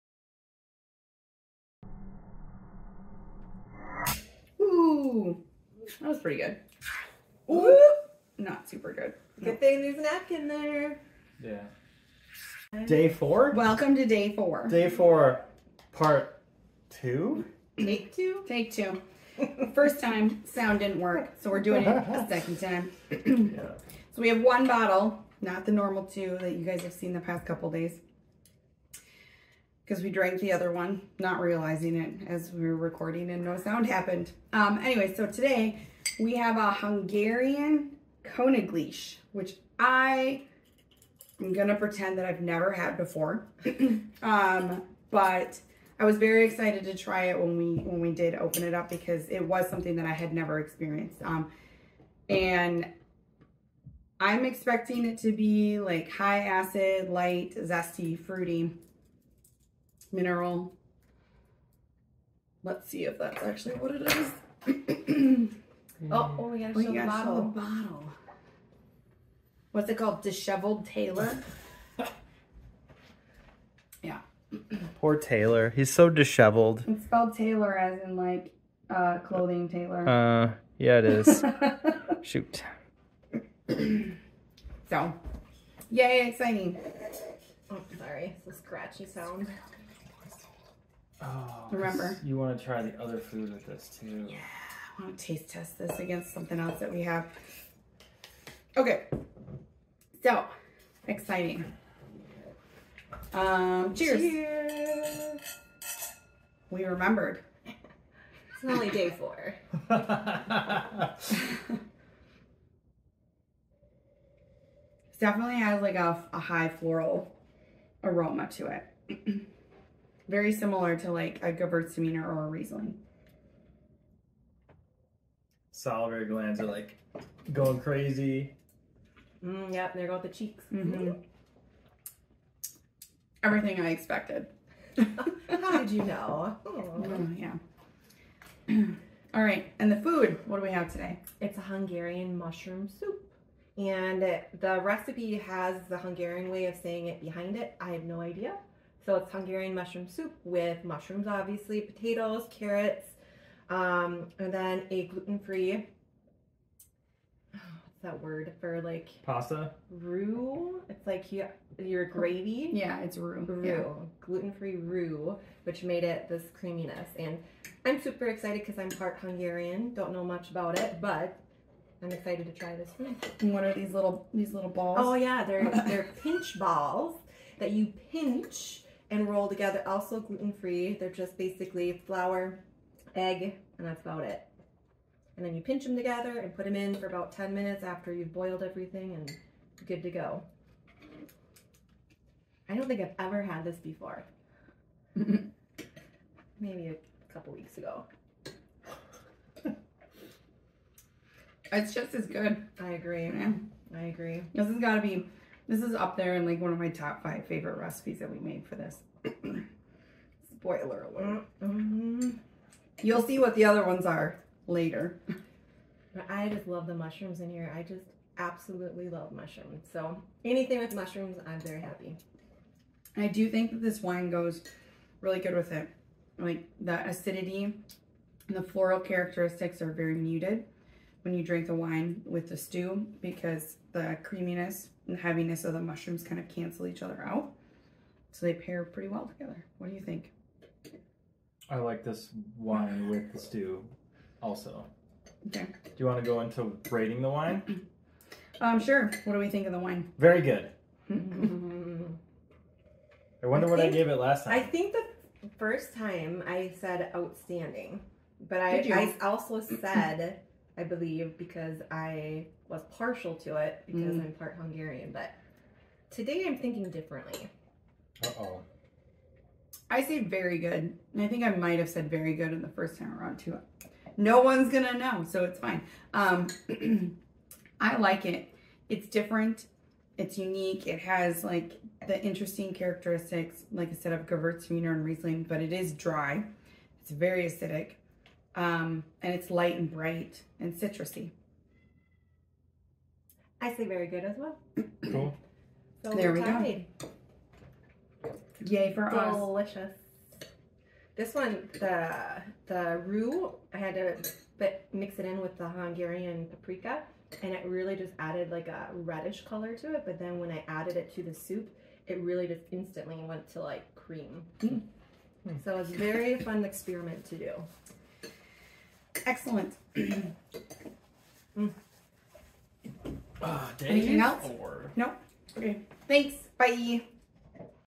Costco Wine Advent Calendar 2022 Ep4
Konigliche, Hungary - Magyaros gombaleves- Hungarian mushroom soup

The Wine Adventurers welcome you to join them in their exploration of the 2022 Costco Wine Advent Calendar.